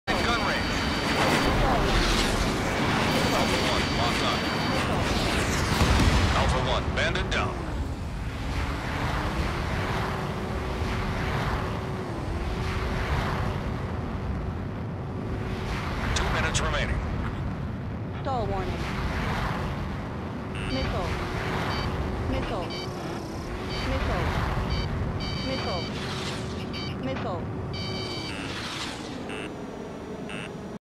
Yo Mig-35D full psm???
look at 0:20

I did not cancel the psm it automatically did that.
No sound effect played as well.